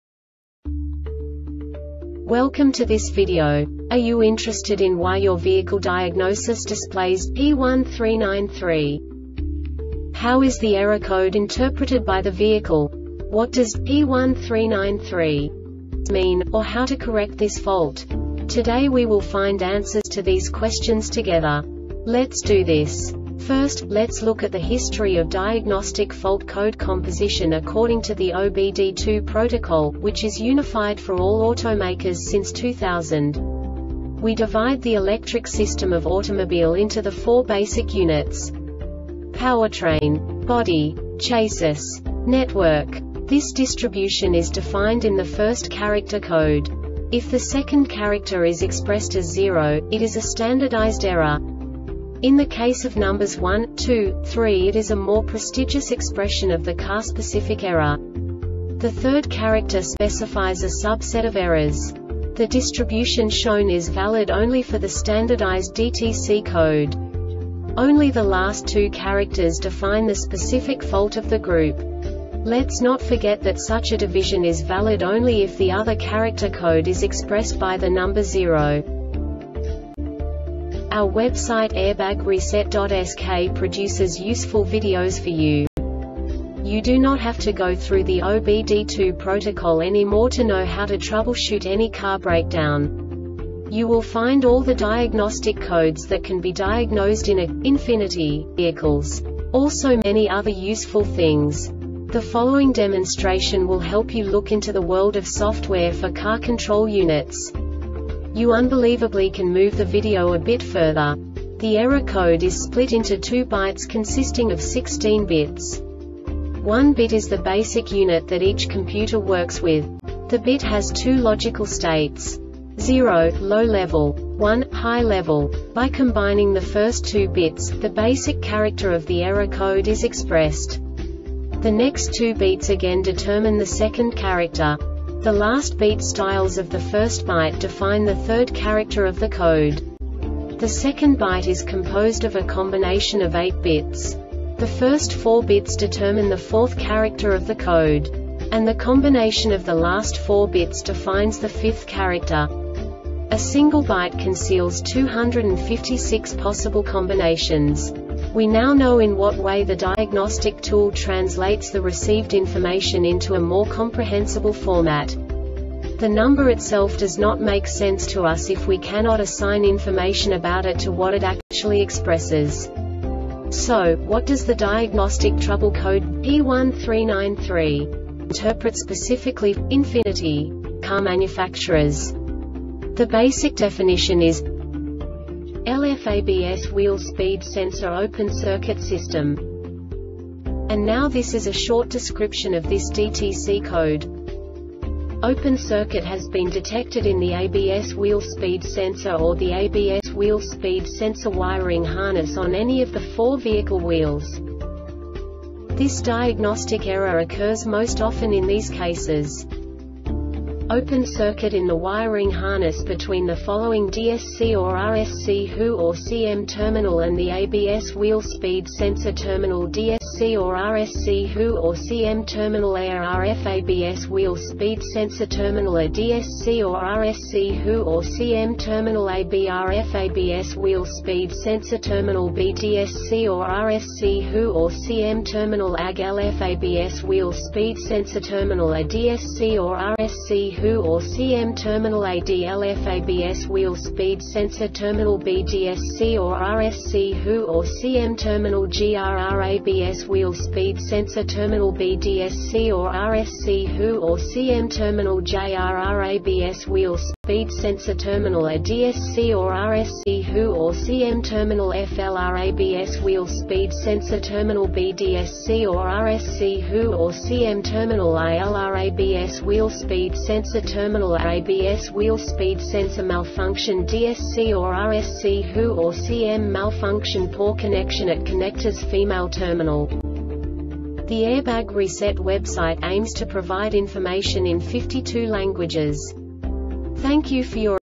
DTC Ford P1393 Short Explanation
Contents:
0:21 Basic DTC analysis according to OBD2 protocol standard.
1:48 Insight into programming
2:58 A diagnostically understandable description of the Diagnostic Trouble Code
3:05 Basic error definition

Make:
Ford
Code:
P1393
Definition:
LF ABS Wheel-Speed Sensor (Open Circuit) System
Description:
Open circuit has been detected in the ABS wheel-speed sensor or the ABS wheel-speed sensor wiring harness on any of the four vehicle wheels
Cause: